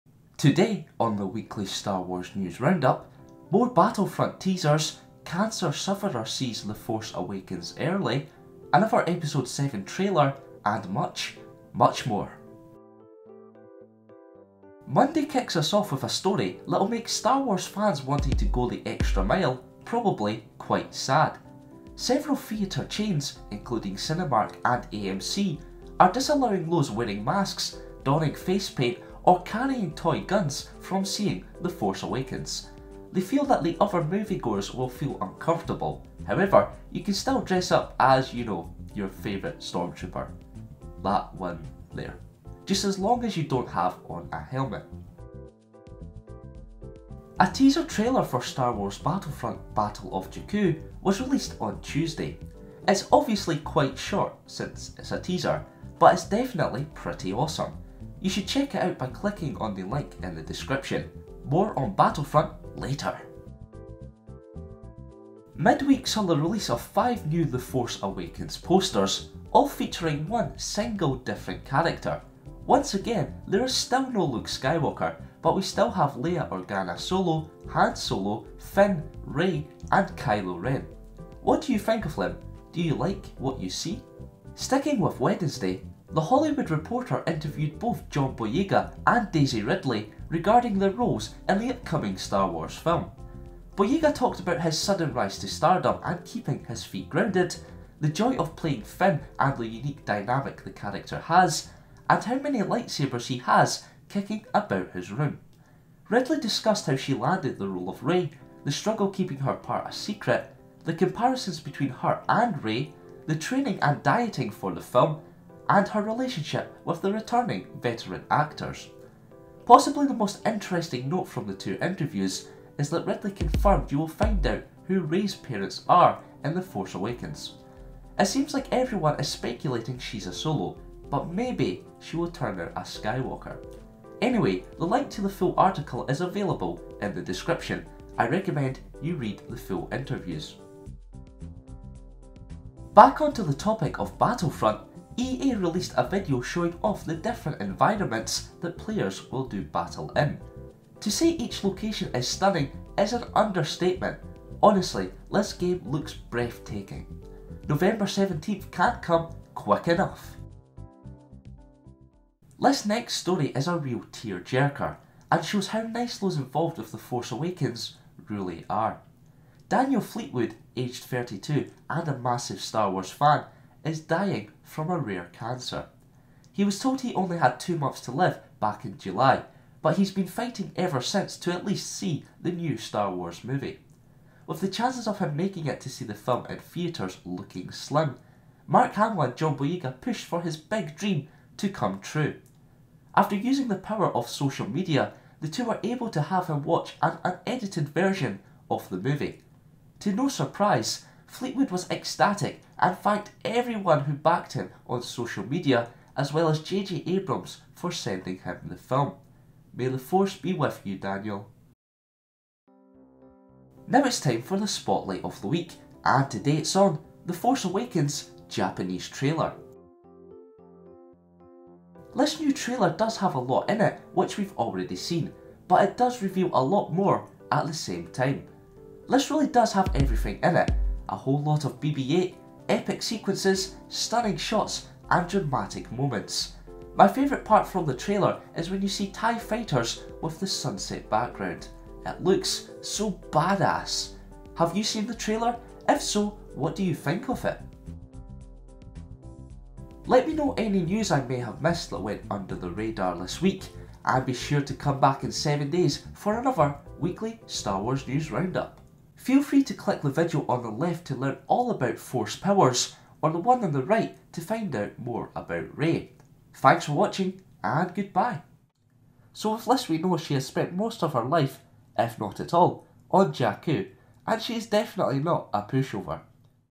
New TFA Trailer! - Weekly Star Wars News Round-up #9
Today on the Weekly Star Wars News Round-up:
More Battlefront teasers, cancer sufferer sees The Force Awakens early, another Episode VII trailer and much, much more!